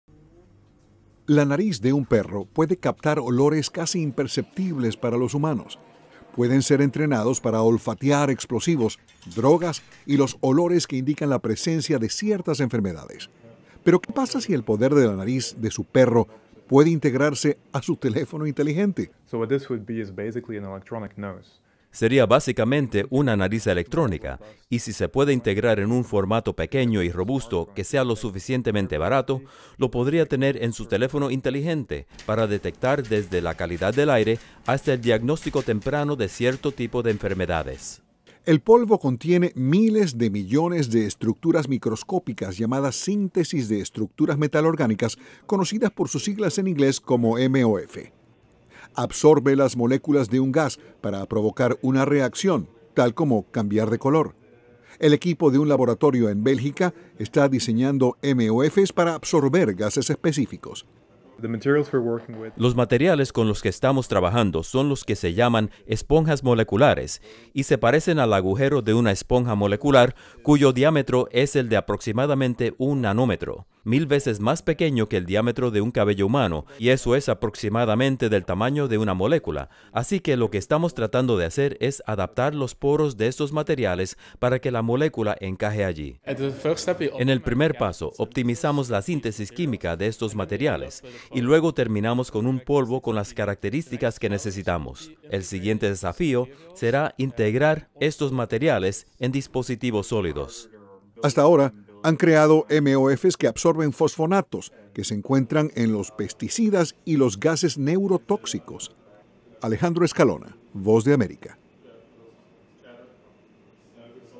Nariz electrónica
Investigadores están desarrollando una sensitiva "nariz electrónica" que puede detectar pesticidas y gases nerviosos, aún en concentraciones muy bajas, usando sustancias que llaman "esponjas microscópicas".